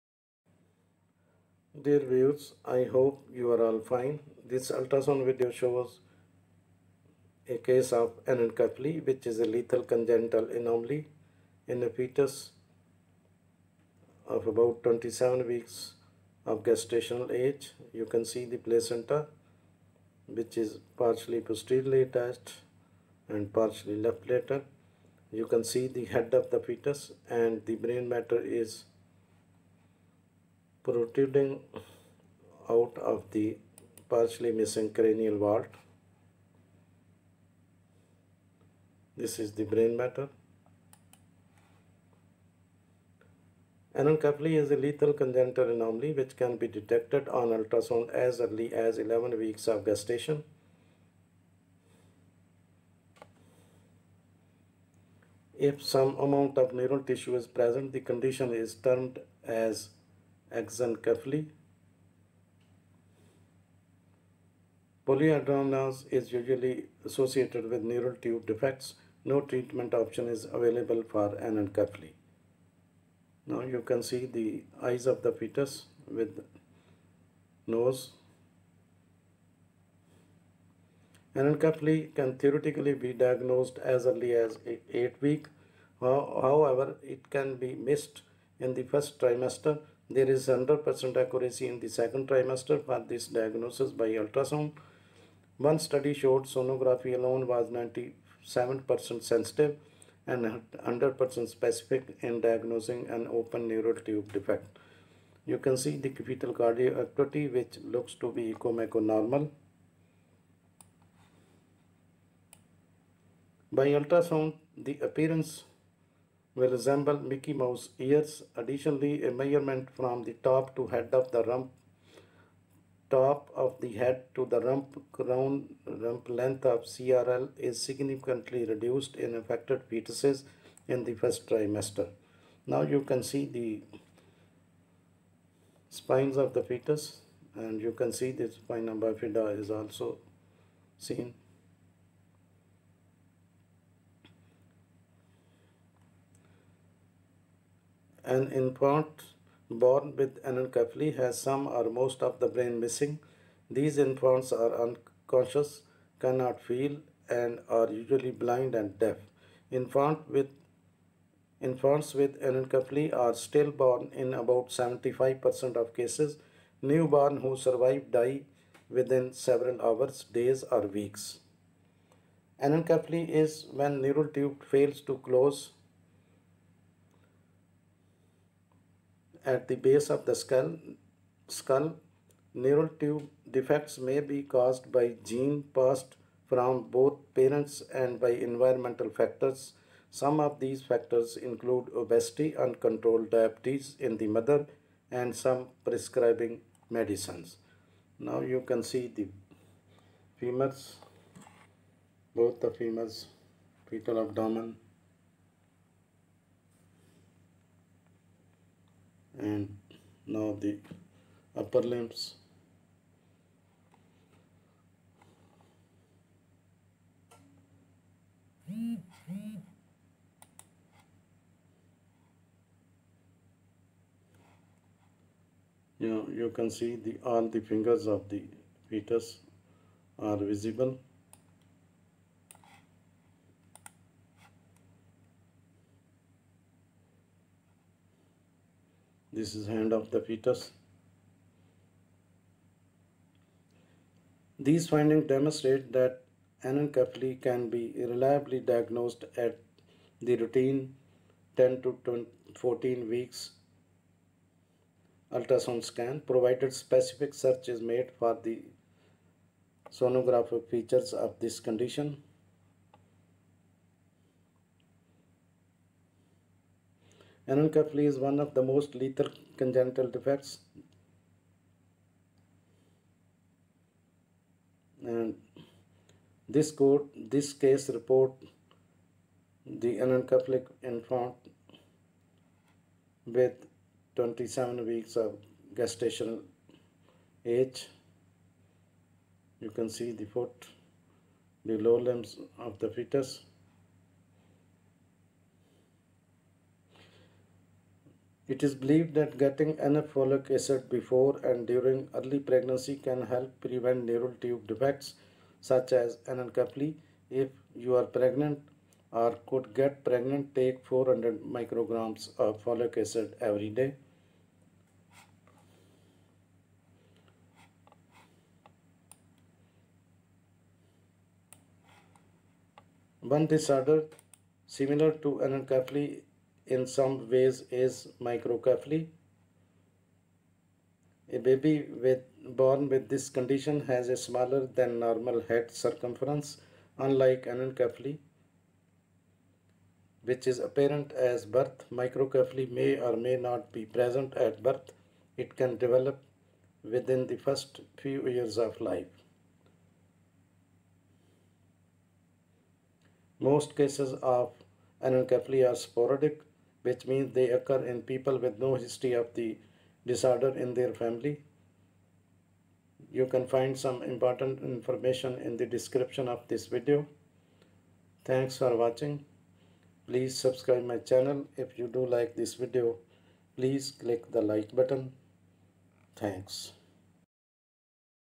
Ultrasound Video showing Anencephaly a lethal congenital anomaly.
This video shows Anencephaly a lethal congenital anomaly.
Anencephaly is a lethal congenital anomaly that can be detected on ultrasound as early as 11 weeks of gestation. If some amount of neural tissue is present, the condition is termed exencephaly. Polyhydramnios is usually associated with neural tube defects. No treatment option is available for anencephaly.
Anencephaly can theoretically be diagnosed as early as 8 weeks; however, it can be missed in the first trimester. There is 100% accuracy in the second trimester for this diagnosis by ultrasound. One study showed sonography alone was 97% sensitive and 100% specific in diagnosing an open neural tube defect.
By ultrasound, the appearance will resemble “Mickey Mouse ears.” Additionally, a measurement from the top of the head to the rump (crown-rump length or CRL) is significantly reduced in affected fetuses in the first trimester.
An infant born with anencephaly has some or most of the brain missing. These infants are unconscious, cannot feel, and are usually blind and deaf. Infants with anencephaly are stillborn in about 75 percent of cases. Newborns who survive die within several hours, days, or weeks.
Anencephaly is when the neural tube fails to close at the base of the skull. Neural tube defects may be caused by genes passed on from both parents and by environmental factors. Some of these factors include obesity, uncontrolled diabetes in the mother, and some prescription medicines.
These findings demonstrate that anencephaly can be reliably diagnosed at the routine 10-14-week ultrasound scan, provided a specific search is made for the sonographic features for this condition.
Anencephaly is one of the most lethal congenital defects. This case report is of an anencephalic infant who lived to 28 months of life and defies current literature.
Getting enough folic acid before and during early pregnancy can help prevent neural tube defects, such as anencephaly. If you are pregnant or could get pregnant, take 400 micrograms (mcg) of folic acid every day.
One disorder similar to anencephaly in some ways is microcephaly. A baby born with this condition has a smaller-than-normal head circumference. Unlike anencephaly, which is apparent at birth, microcephaly may or may not be present at birth. It can develop within the first few years of life.
Most cases of anencephaly are sporadic, which means they occur in people with no history of the disorder in their family. A small percentage of cases have been reported to run in families; however, the condition does not have a clear pattern of inheritance.